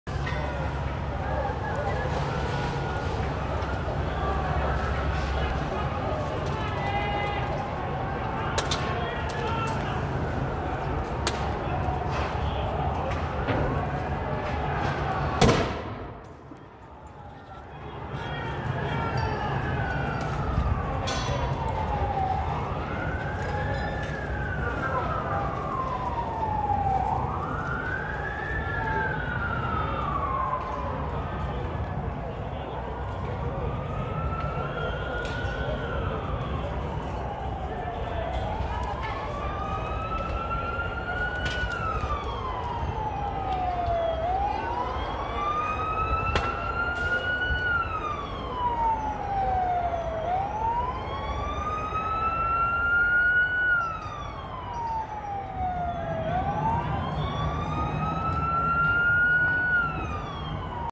Carbineros clear out protesters near field hospitol for the wounded
A field hospitol is setup to takw care of the wounded. Police clear put protesters from around hospitol